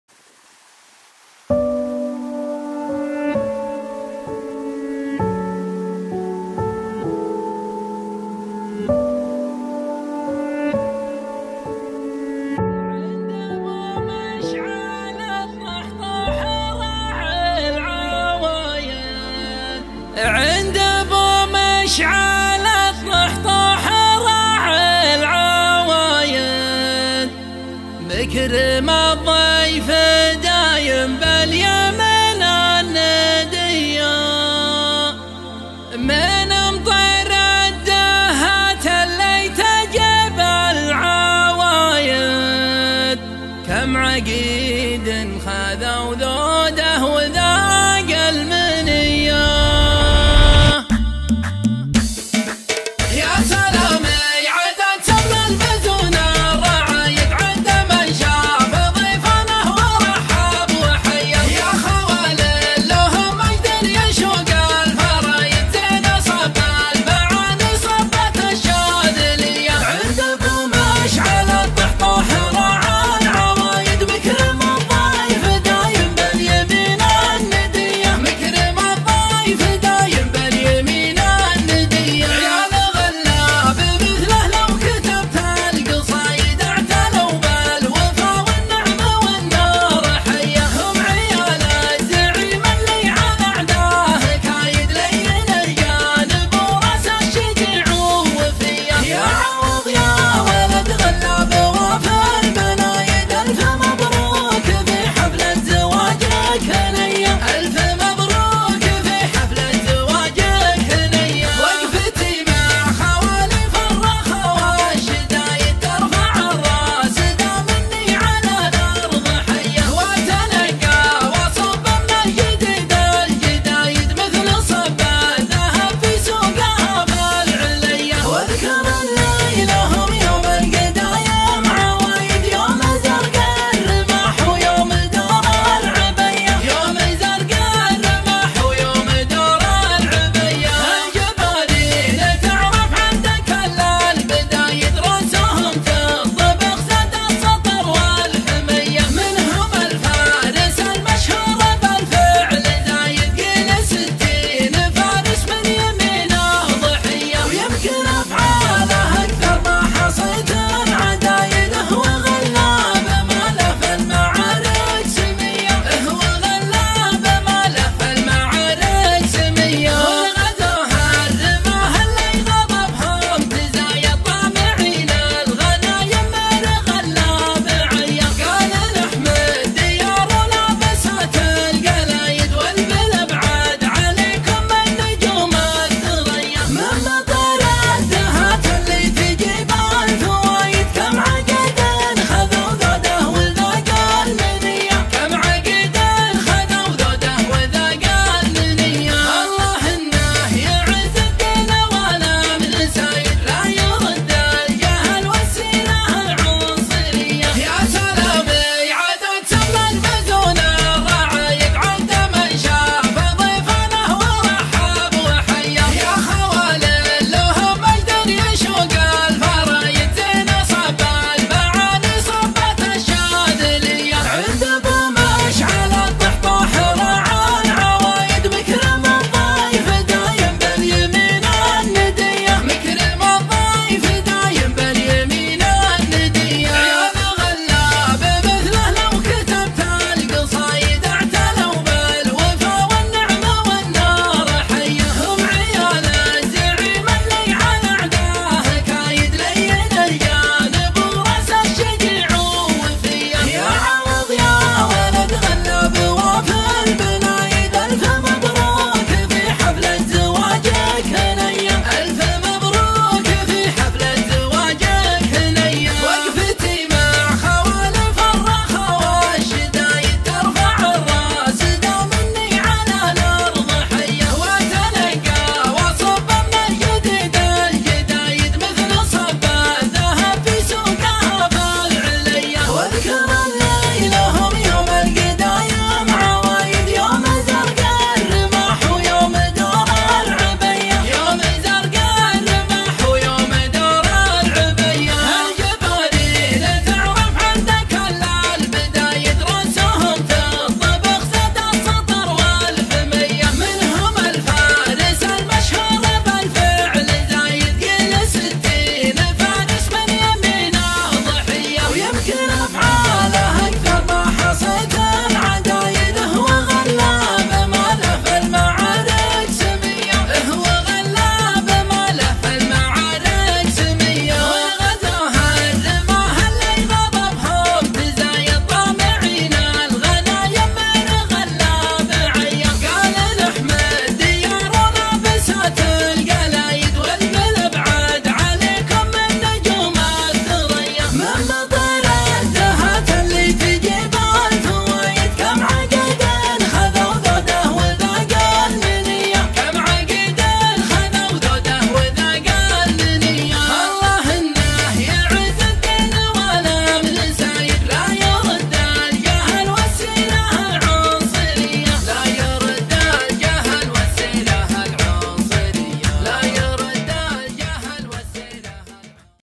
Saudi Arabian Traditional Wedding | Video 5
This is the men section in a Saudi Arabian wedding ceremony. Women and men celebrate in separate halls due to religious and cultural reasons (e.g. women want to dress nicely without the need to wear hijab). Anyways, the groom will join his bride later in women hall after getting greeted and congratulated by the male guests and filmming will continue but that part of the wedding won't be published online for the same reasons.
جميع حقوق النسخ والنشر تنتمي إلى أصحابها.